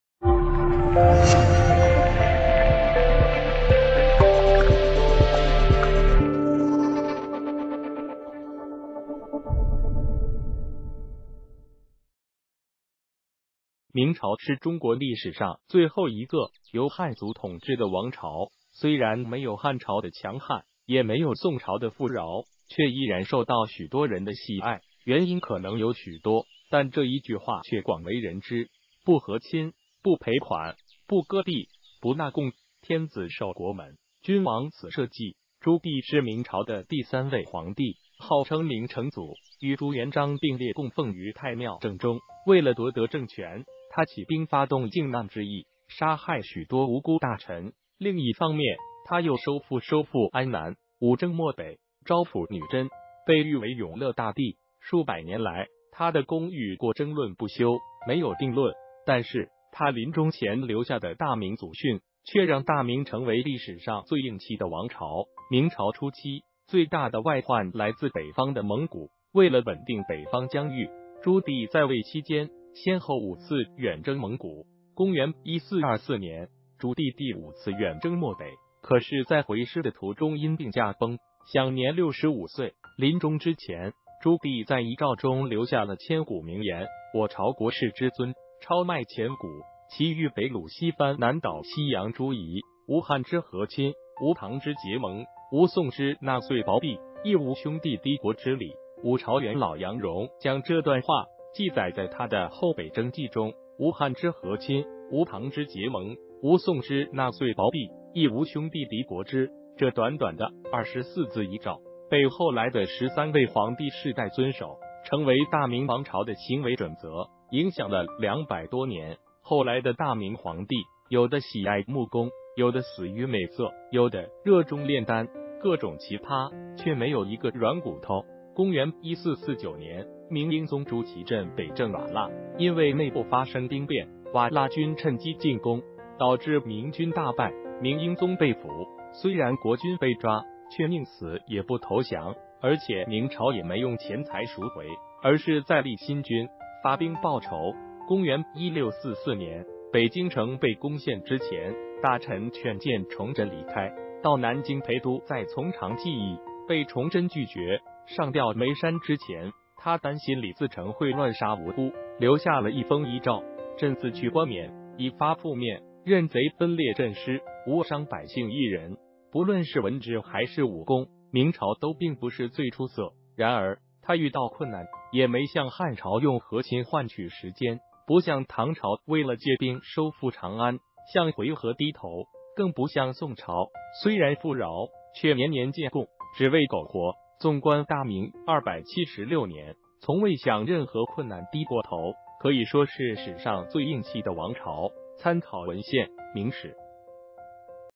朱棣临终留下祖训，仅仅二十四字，让明朝成为最有骨气的王朝
谢谢！朱棣临终留下祖训，仅仅二十四字，让明朝成为最有骨气的王朝